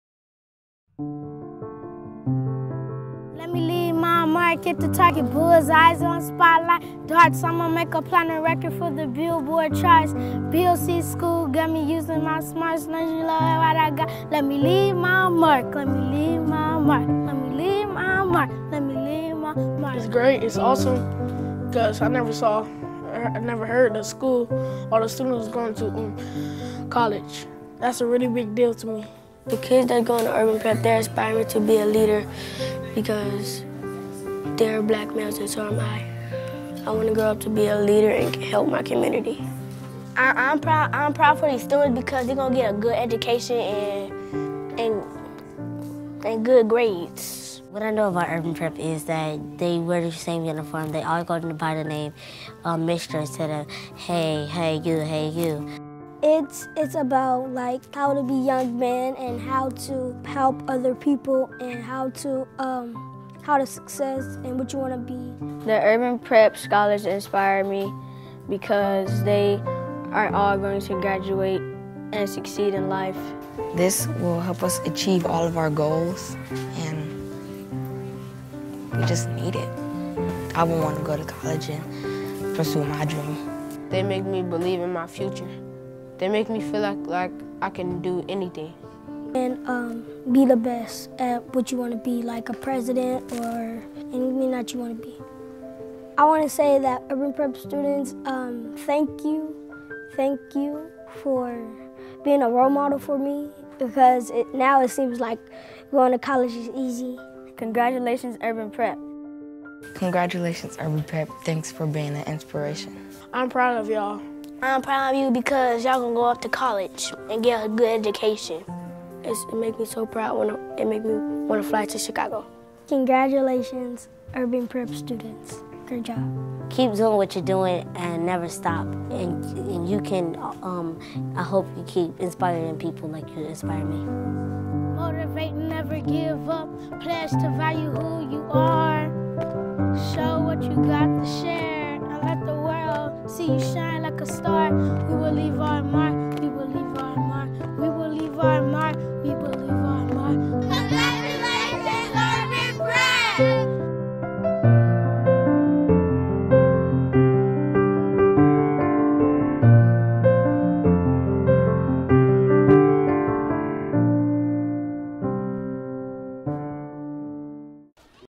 Barack Obama Charter School Students Congratulate Urban Prep Graduates 2013
Elementary school students from Barack Obama Charter School sent a video message of congratulations to Urban Prep graduates that was shown during Commencement 2013.